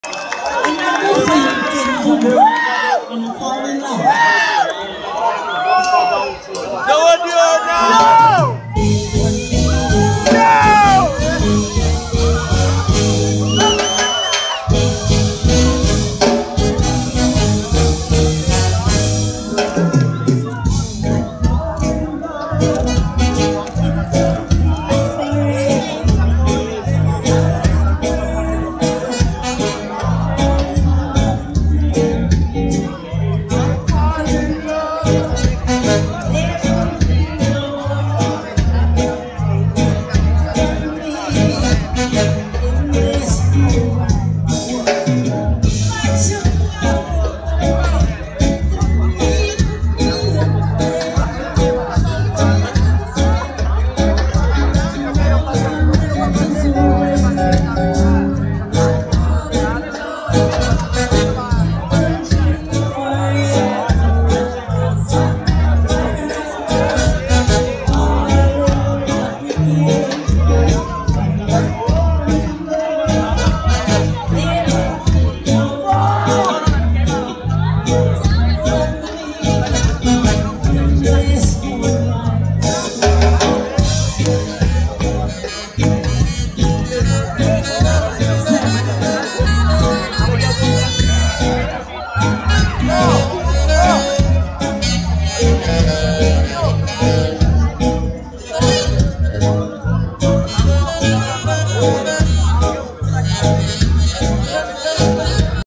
The Skatalites @ Vale do Anhangabaú, São Paulo 12/02/2018, Parte 4
Inesquecível Apresentação da lendária Doreen Schaffer juntamente com a banda the Skatalites no CarnaSka, durante o carnaval de São Paulo em 2018